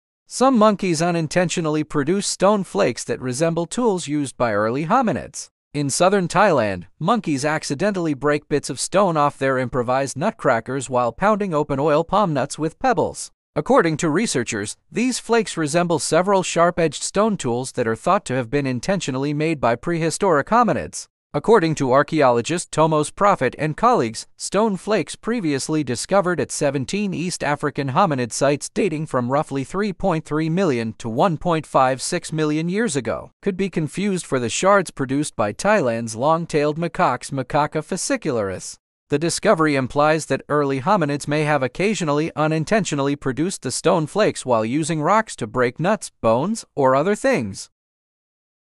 Monkeys Accidentally Make Stone Flakes that Resemble Ancient Hominid Tools.
Some monkeys unintentionally produce stone flakes that resemble tools used by early hominids.

In southern Thailand, monkeys accidentally break bits of stone off their improvised nutcrackers while pounding open oil palm nuts with pebbles. According to researchers, these flakes resemble several sharp-edged stone tools that are thought to have been intentionally made by prehistoric hominids.
According to archaeologist Tomos Proffitt and colleagues, stone flakes previously discovered at 17 East African hominid sites dating from roughly 3.3 million to 1.56 million years ago could be confused for the shards produced by Thailand's long-tailed macaques (Macaca fascicularis). The discovery implies that early hominids may have occasionally unintentionally produced the stone flakes while using rocks to break nuts, bones, or other things.